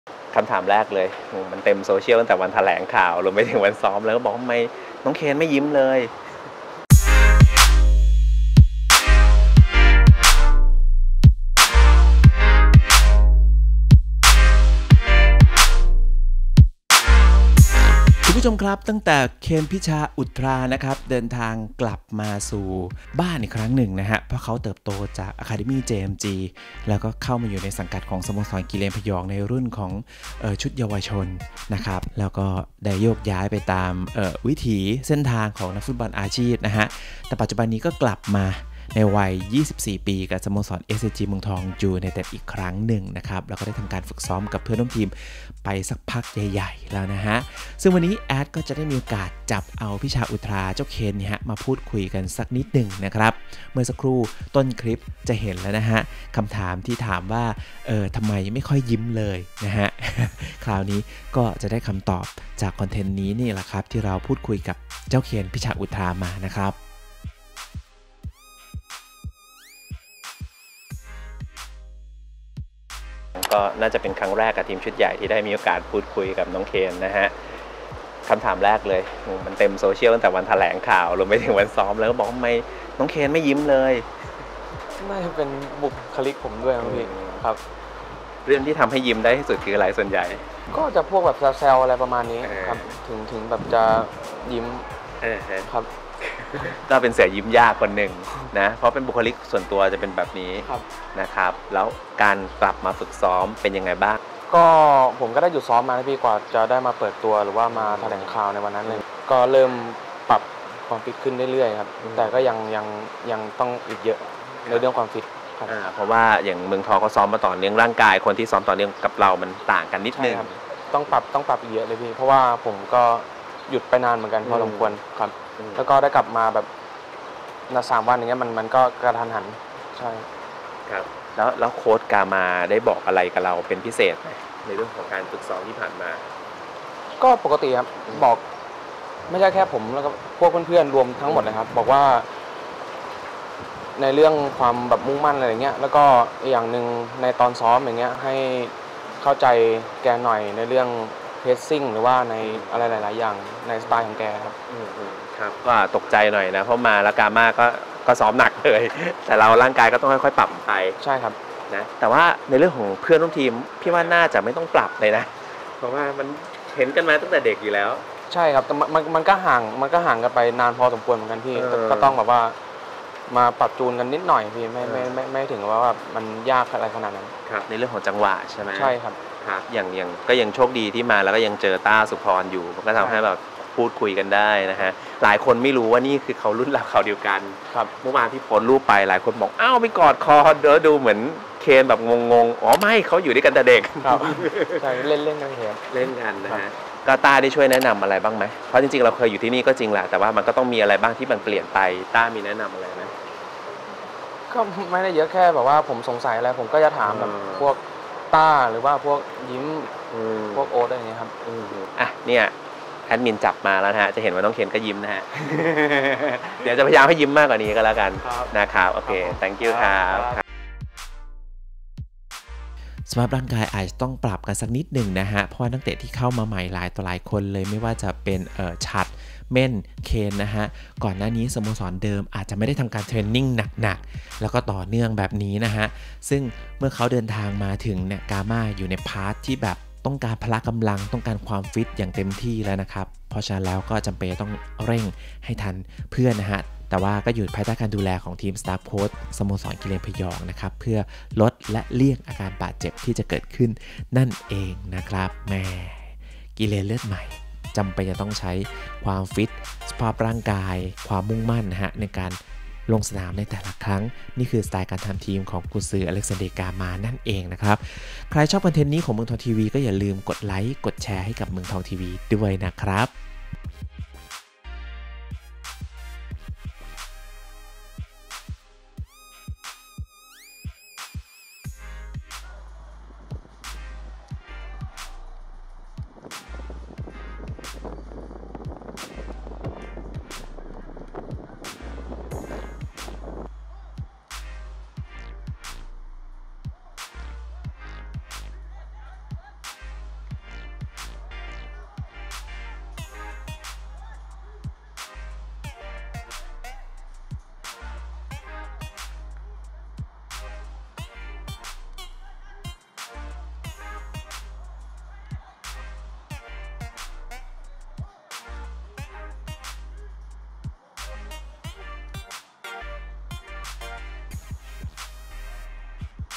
สัมภาษณ์เคน - พิชา อุทรา ครั้งแรก : " คำถามที่แฟนคลับอยากรู้ ทำไมเคนไม่คอยยิ้ม แอดฯถามมาให้แล้ว "
เมืองทองทีวี : สัมภาษณ์เคน - พิชา อุทรา ครั้งแรก : " คำถามที่แฟนคลับอยากรู้ ทำไมเคนไม่คอยยิ้ม แอดฯถามมาให้แล้ว "